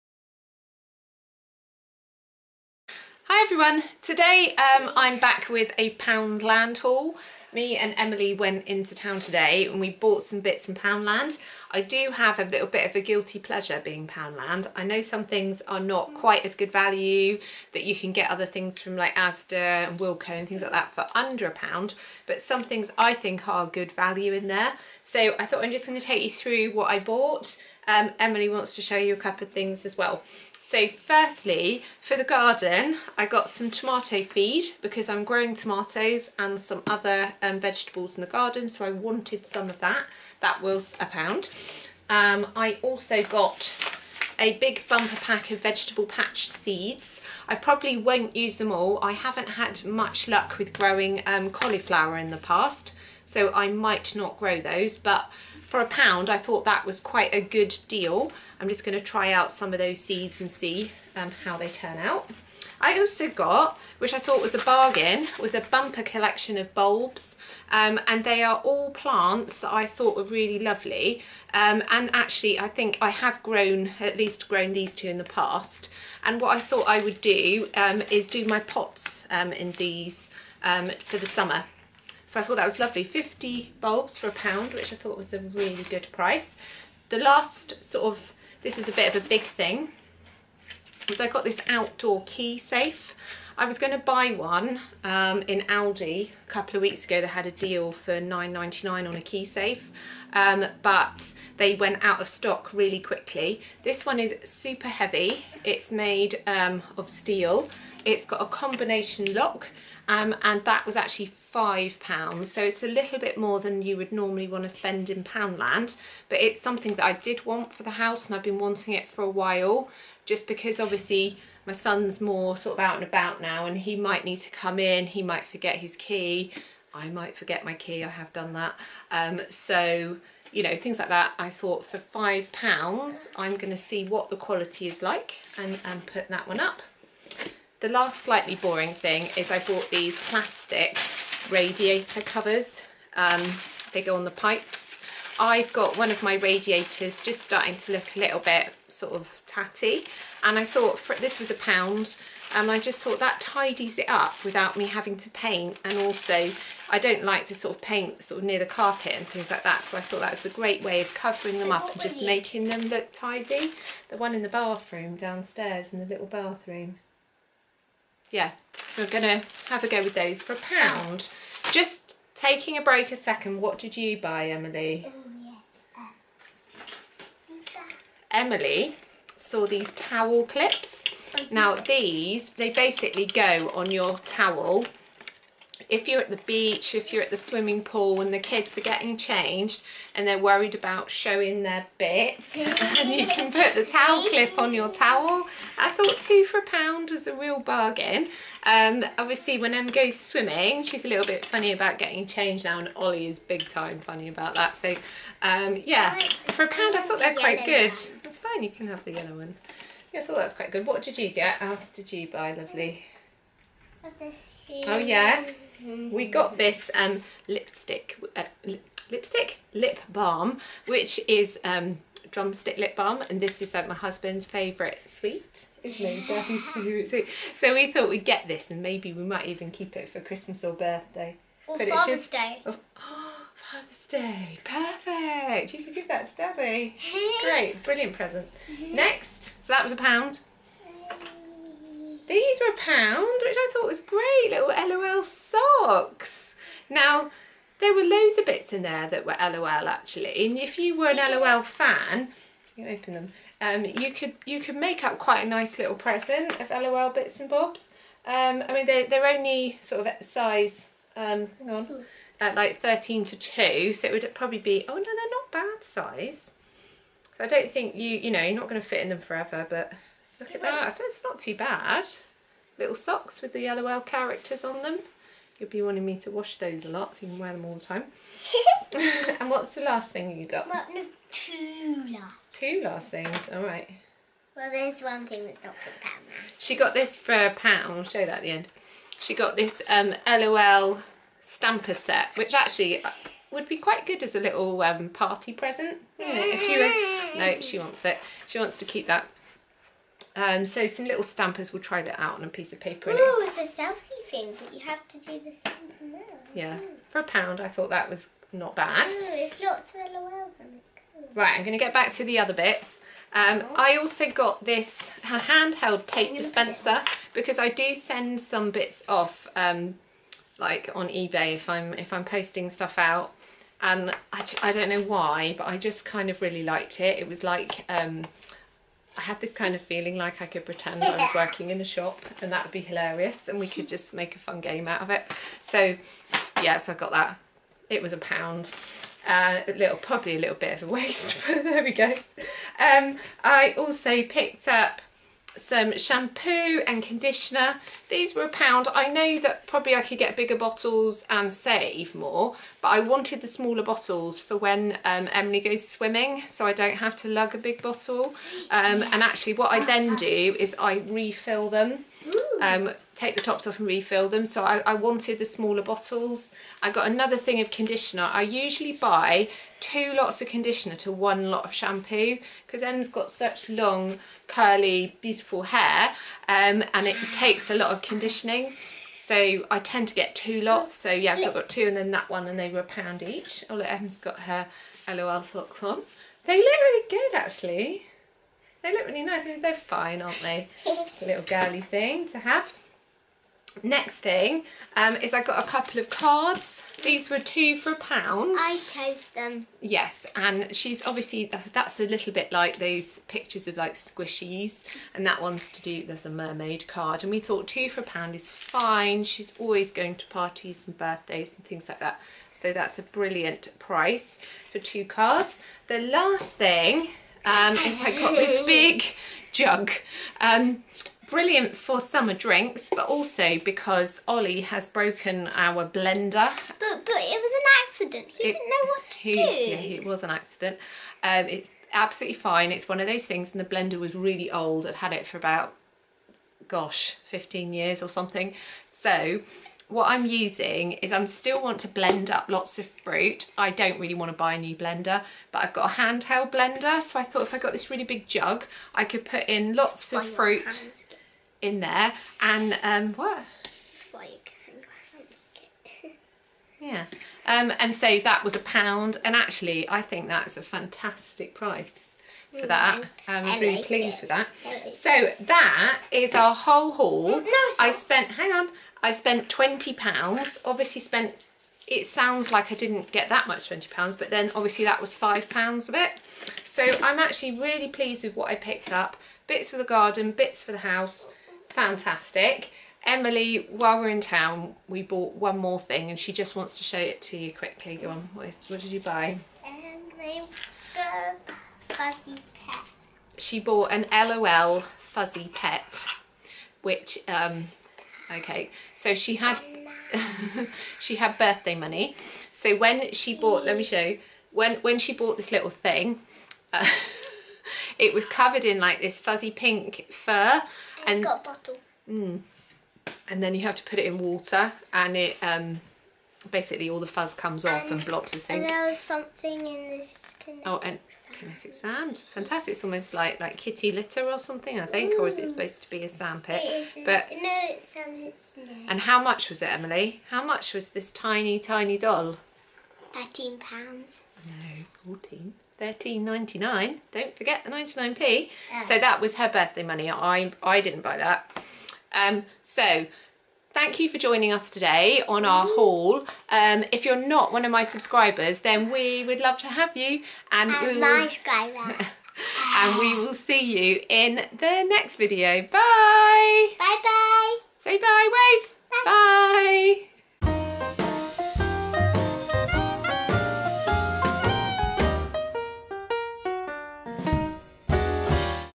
POUNDLAND HAUL | WHAT"S NEW IN POUNDLAND | HOME AND GARDEN HAUL
This video is my Poundland Haul.  We had a great time picking out lots of things we needed for the home and garden for only £20.